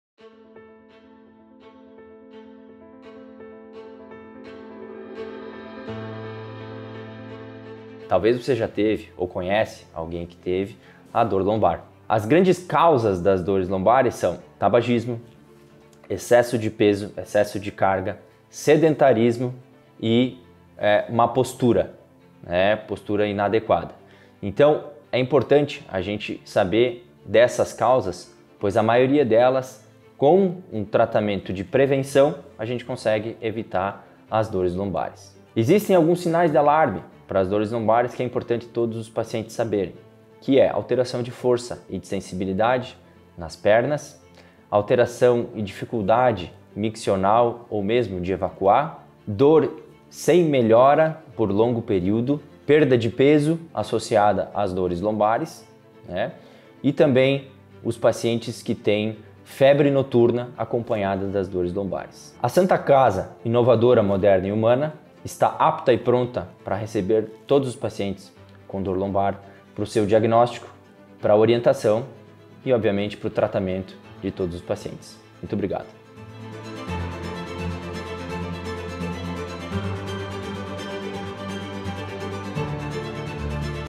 Como Evitar a Dor Lombar?
Assista o vídeo e descubra quais são as causas e sintomas das dores lombar (lombalgia).